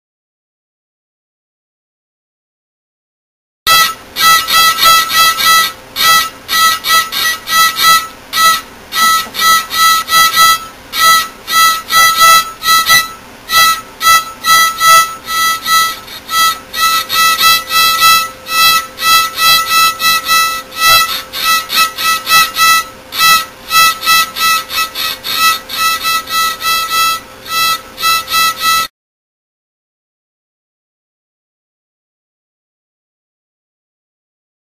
Violin
Nate practicing his E string by himself...